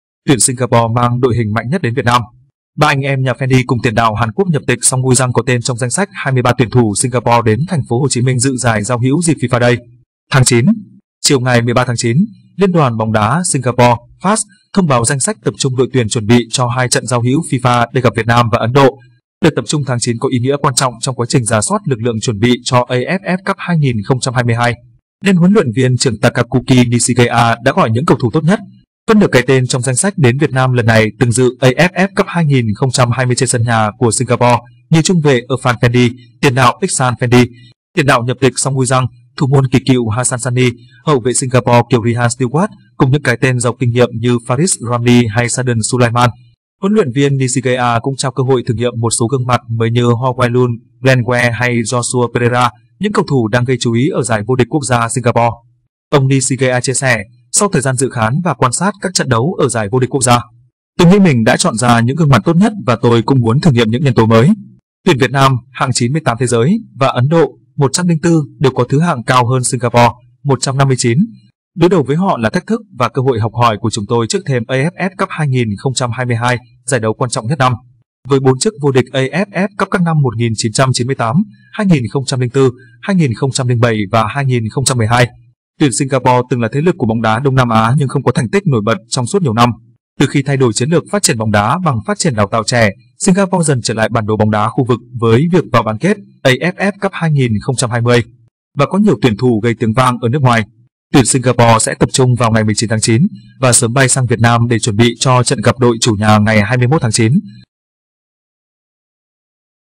Tuyển Singapore mang đội hình mạnh nhất đến Việt Nam
Ba anh em nhà Fandi cùng tiền đạo Hàn Quốc nhập tịch Song Ui-young có tên trong danh sách 23 tuyển thủ Singapore đến TP.HCM dự giải giao hữu dịp FIFA Days tháng 9.Tuyển Singapore sẽ tập trung vào ngày 19/9 và sớm bay sang Việt Nam để chuẩn bị cho trận gặp đội chủ nhà ngày 21/9.
HLV Nishigaya cũng trao cơ hội thử nghiệm một số gương mặt mới như Ho Wai Loon, Glenn Kweh hay Joshua Pereira, những cầu thủ đang gây chú ý ở giải vô địch quốc gia Singapore.
Phân nửa cái tên trong danh sách đến Việt Nam lần này từng dự AFF Cup 2020 trên sân nhà của Singapore, như trung vệ Irfan Fandi, tiền đạo Ikhsan Fandi, tiền đạo nhập tịch Song Ui-young, thủ môn kỳ cựu Hassan Sunny, hậu vệ Singapore kiều Ryhan Stewart cùng những cái tên giàu kinh nghiệm như Faris Ramli hay Shahdan Sulaiman.
 Lứa cầu thủ mới của tuyển Singapore hiện tại với mũi nhọn Ikhsan Fandi (số 9) từng gây khó khăn cho U23 Việt Nam ở vòng bảng SEA Games 2019 ở Philippines. Ảnh: Thuận Thắng.
Chiều 13/9, Liên đoàn Bóng đá Singapore (FAS) thông báo danh sách tập trung đội tuyển chuẩn bị cho hai trận giao hữu FIFA Days gặp Việt Nam và Ấn Độ. Đợt tập trung tháng 9 có ý nghĩa quan trọng trong quá trình rà soát lực lượng chuẩn bị cho AFF Cup 2022 nên HLV trưởng Takayuki Nishigaya đã gọi những cầu thủ tốt nhất.
Ông Nishigaya chia sẻ: “Sau thời gian dự khán và quan sát các trận đấu ở giải VĐQG, tôi nghĩ mình đã chọn ra những gương mặt tốt nhất và tôi cũng muốn thử nghiệm những nhân tố mới. Tuyển Việt Nam (hạng 98 thế giới) và Ấn Độ (104) đều có thứ hạng cao hơn Singapore (159). Đối đầu với họ là thách thức và cơ hội học hỏi của chúng tôi trước thềm AFF Cup 2022, giải đấu quan trọng nhất năm”.
Với 4 chức vô địch AFF Cup các năm 1998, 2004, 2007 và 2012, tuyển Singapore từng là thế lực của bóng đá Đông Nam Á nhưng không có thành tích nổi bật trong suốt nhiều năm. Từ khi thay đổi chiến lược phát triển bóng đá bằng phát triển đào tạo trẻ, Singapore dần trở lại bản đồ bóng đá khu vực với việc vào bán kết AFF Cup 2020 và có nhiều tuyển thủ gây tiếng vang ở nước ngoài.